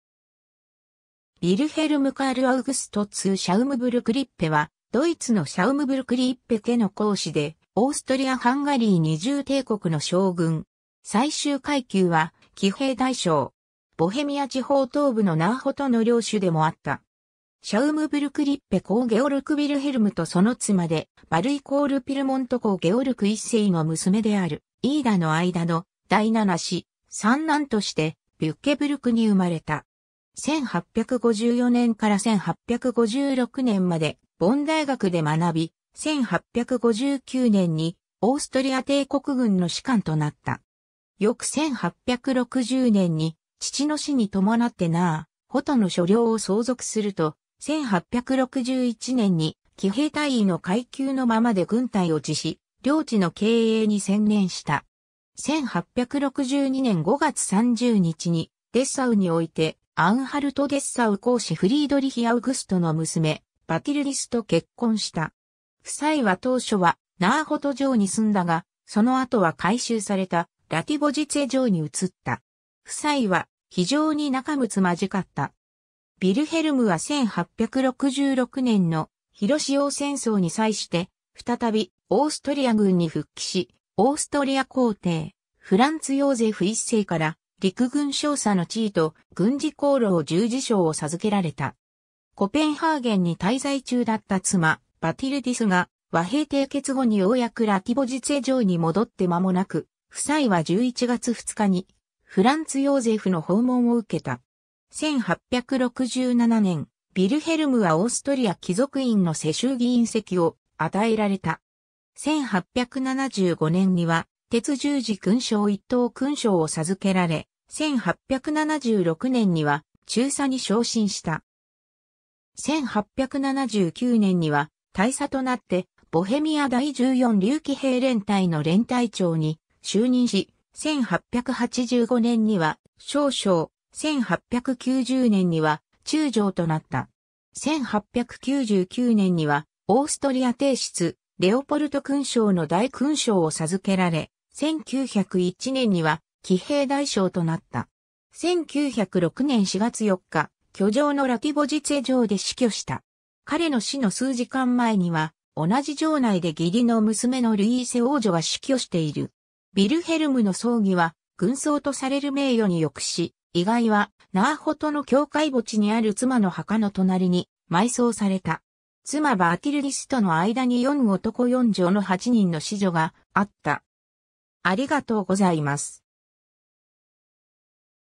ヴィルヘルム・ツー・シャウムブルク＝リッペ
オーストリア＝ハンガリー帝国の軍人
ヴィルヘルム・カール・アウグスト・ツー・シャウムブルク＝リッペ（Wilhelm Karl August zu Schaumburg-Lippe, 1834年12月12日 - 1906年4月4日）は、ドイツのシャウムブルク＝リッペ家の侯子で、オーストリア＝ハンガリー二重帝国の将軍。
最終階級は騎兵大将。
ボヘミア地方東部のナーホトの領主でもあった。
 シャウムブルク＝リッペ侯ゲオルク・ヴィルヘルムとその妻でヴァルデック＝ピルモント侯ゲオルク1世の娘であるイーダの間の第7子、三男としてビュッケブルクに生まれた。
1854年から1856年までボン大学で学び、1859年にオーストリア帝国軍の士官となった。
翌1860年に父の死に伴ってナーホトの所領を相続すると、1861年に騎兵大尉の階級のままで軍隊を辞し、領地の経営に専念した。
1862年5月30日にデッサウにおいて、アンハルト＝デッサウ公子フリードリヒ・アウグストの娘バティルディスと結婚した。
夫妻は当初はナーホト城に住んだが、その後は改修されたラティボジツェ城に移った。
夫妻は非常に仲睦まじかった。
 ヴィルヘルムは1866年の普墺戦争に際して再びオーストリア軍に復帰し、オーストリア皇帝フランツ・ヨーゼフ1世から陸軍少佐の地位と軍事功労十字章 (Militärverdienstkreuz) を授けられた。
コペンハーゲンに滞在中だった妻バティルディスが和平締結後にようやくラティボジツェ城に戻ってまもなく、夫妻は11月2日にフランツ・ヨーゼフの訪問を受けた。
1867年、ヴィルヘルムはオーストリア貴族院の世襲議員席を与えられた。
 1875年には鉄十字勲章 (Orden der Eisernen Krone) 1等勲章を授けられ、1876年には中佐に昇進した。
1879年には大佐となってボヘミア第14竜騎兵連隊 (K.u.k. Böhmisches Dragoner-Regiment „Fürst zu Windisch-Graetz“ Nr. 14) の連隊長に就任し、1885年には少将、1890年には中将となった。1899年にはオーストリア帝室レオポルト勲章 (Österreichisch-kaiserlicher Leopold-Orden) の大勲章を授けられ、1901年には騎兵大将となった。 1906年4月4日、居城のラティボジツェ城で死去した。彼の死の数時間前には同じ城内で義理の娘のルイーセ王女（デンマーク王フレゼリク8世の娘）が死去している。ヴィルヘルムの葬儀は軍葬とされる名誉に浴し、遺骸はナーホトの教会墓地にある妻の墓の隣に埋葬された。 妻バティルディスとの間に4男4女の8人の子女があった。